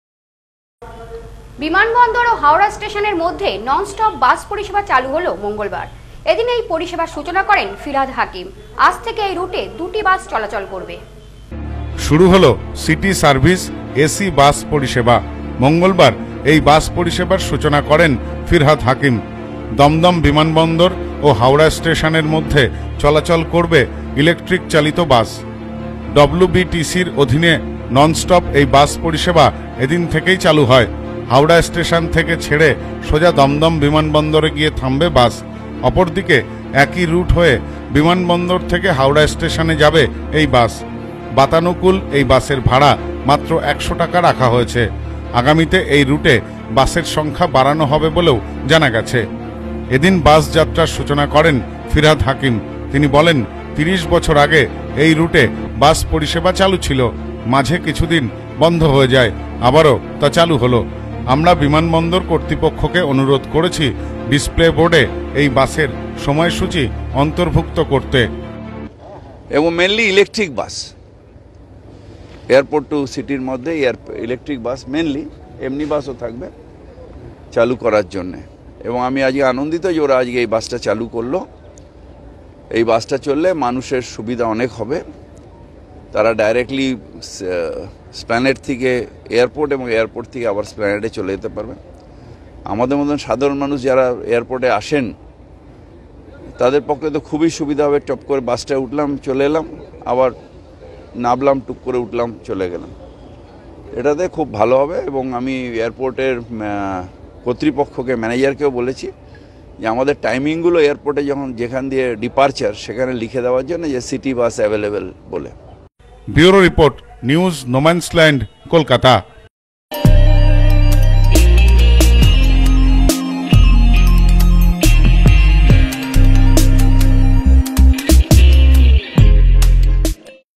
বিমানবন্দর ও হাওড়া স্টেশনের মধ্যে 'ননস্টপ' বাস পরিষেবা চালু৷
বিমানবন্দর ও হাওড়া স্টেশনের মধ্যে 'ননস্টপ' বাস পরিষেবা চালু হলো মঙ্গলবার ৷পরিষেবার সূচনা করেন রাজ্যের মন্ত্রী ফিরহাদ হাকিম ।আজ থেকে দুটি বাস চলবে এই রুটে`